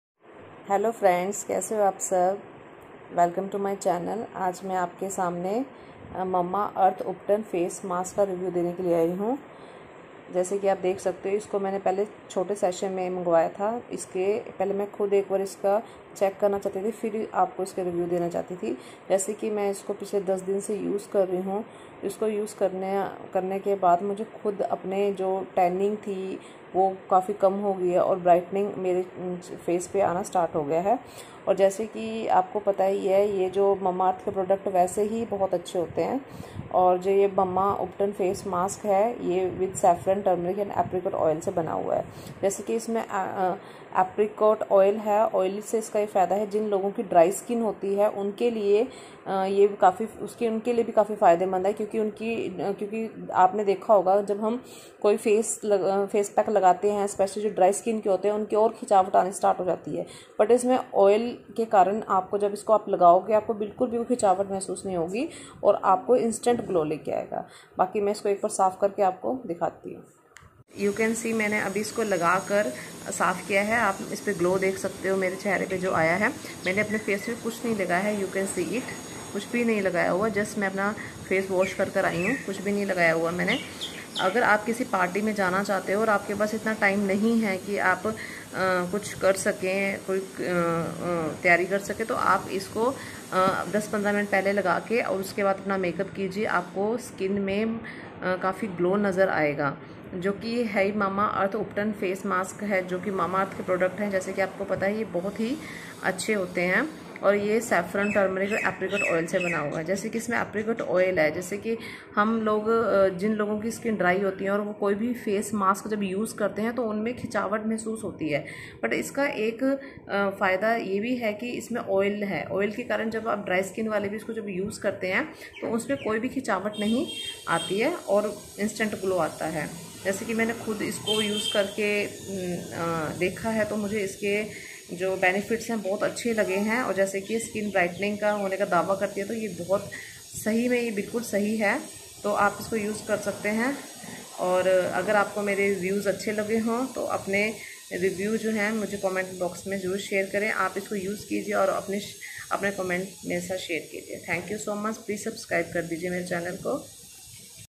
Mamma earth ubtan face mask , instant glow,skin brightenning,
hi Everyone

Mamma earth ubtan face mask is suitable for all skin types.paraben nd  silicone free, Dramatically tested,anti-aging lightens ND brightens skin made with natural ingredients in it.

product link

Mamaearth Ubtan Face Pack Mask For Fairness, Tanning & Glowing Skin With Saffron, Turmeric & Apricot Oil (100 ml)